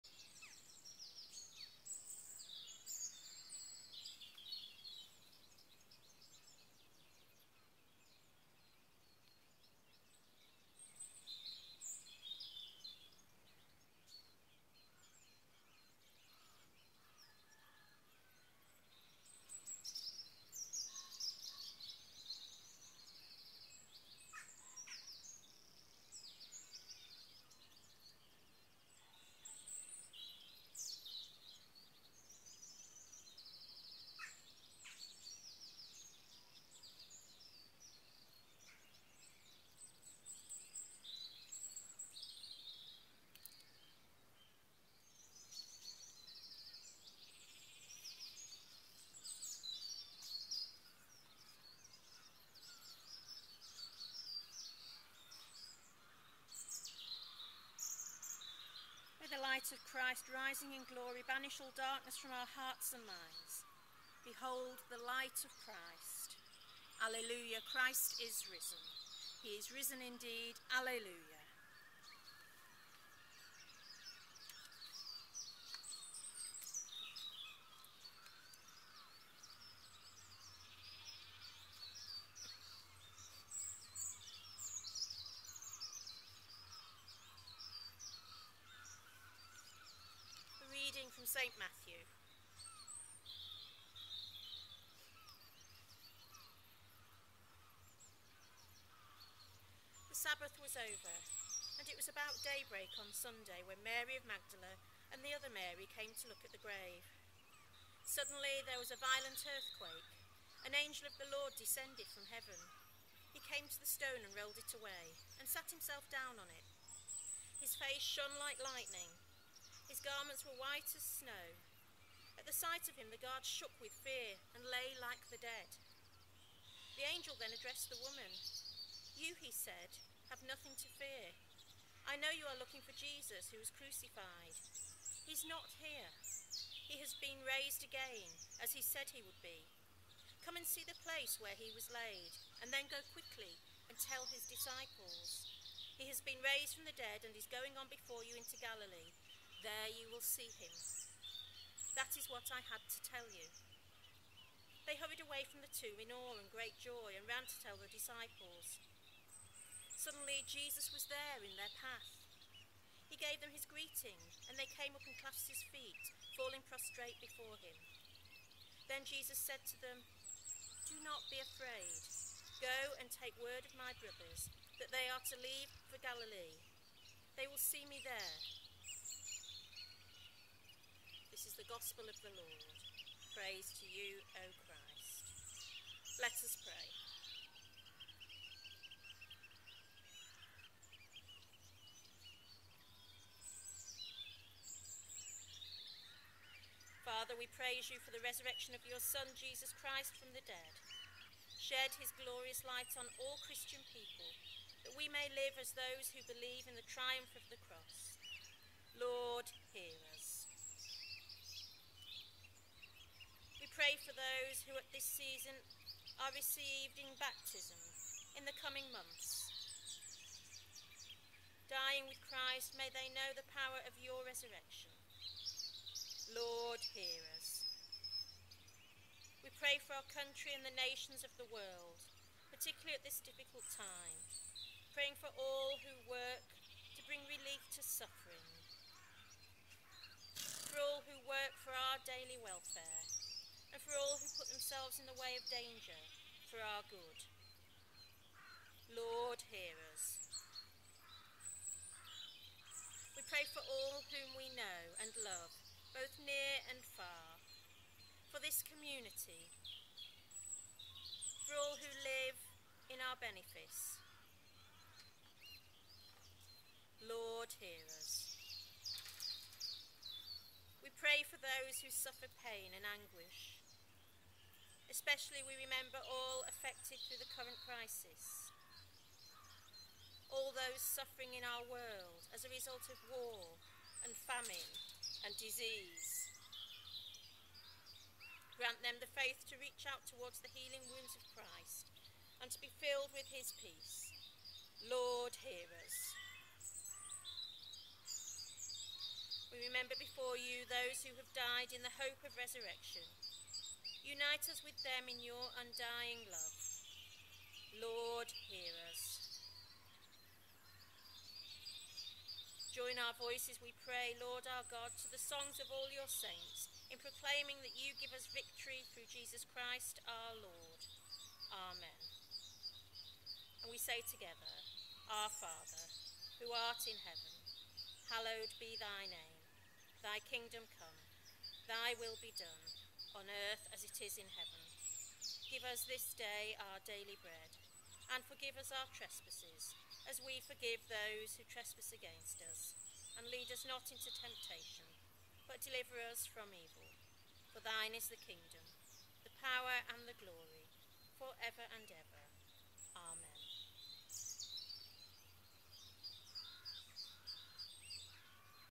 Dawn Easter 2020 - Part 2
Lighting of the Benefice Pascal candles, Reading & prayers for Easter.